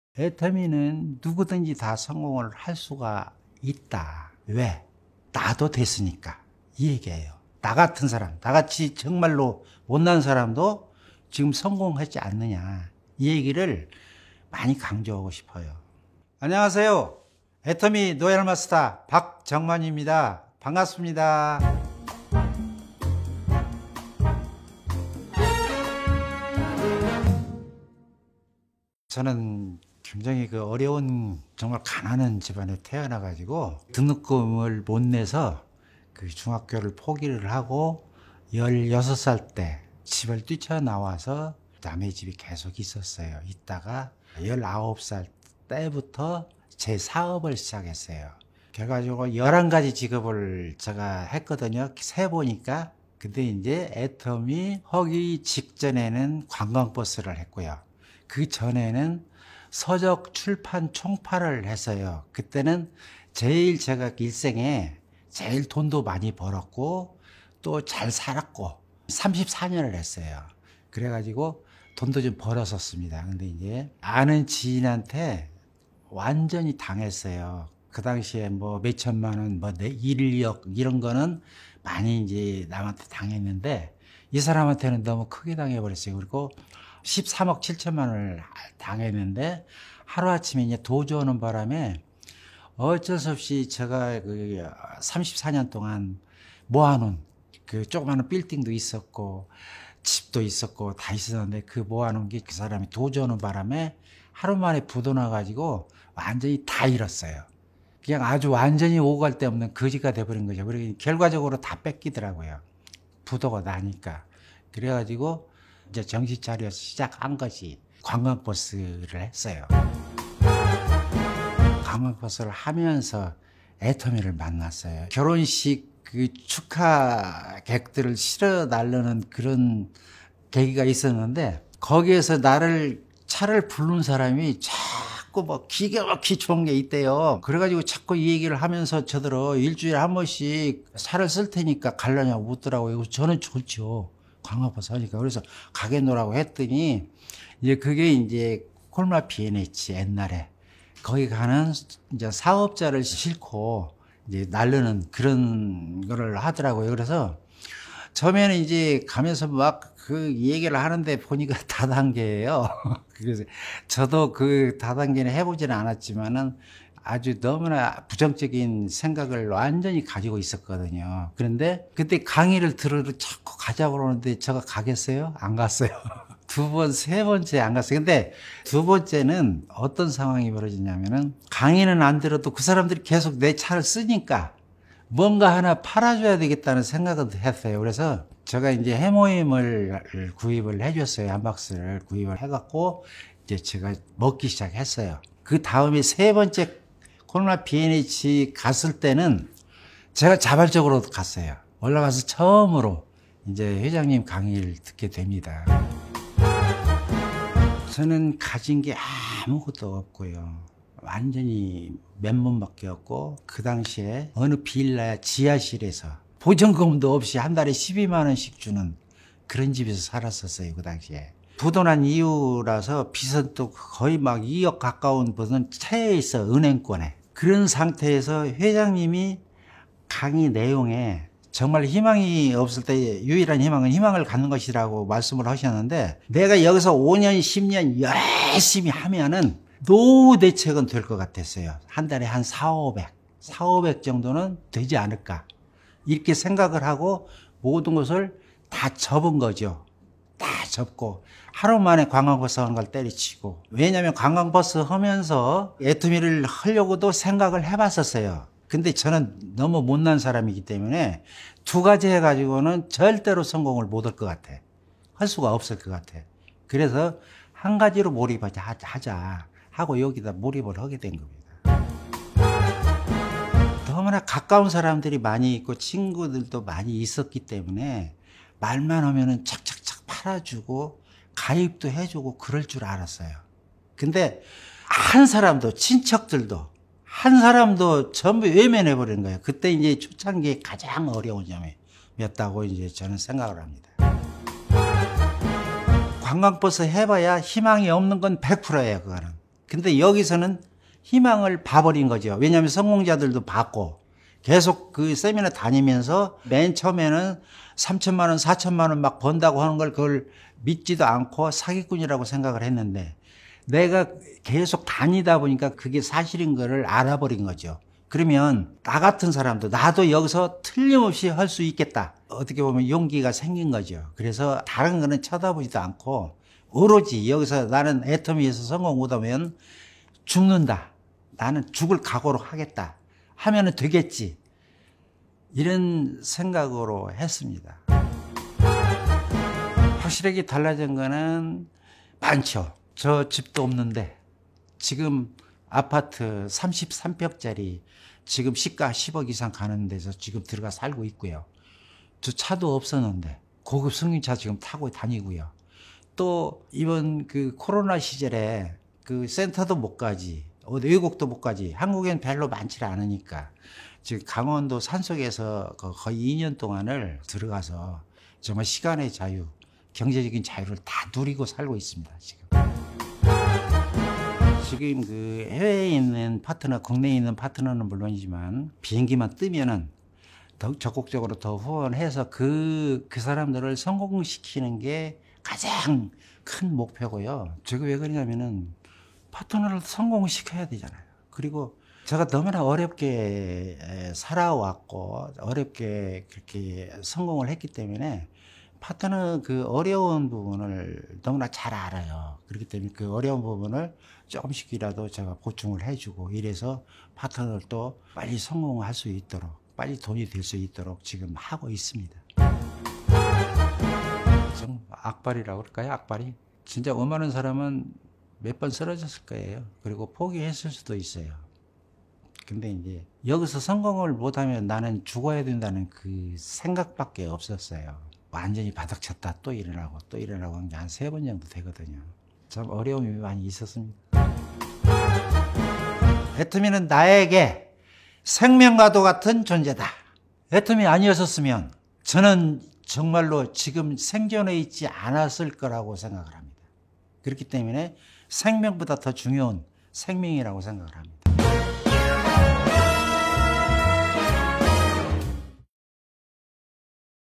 Success Story of A Bus Driver To Atomy Team Warrior Royal Master Jung Man Park Monthly Earn $40,000
1. Like Our Pages on Facebook-Team Warrior Rathore
2. Like Our Pages on Facebook-Atomians Warrior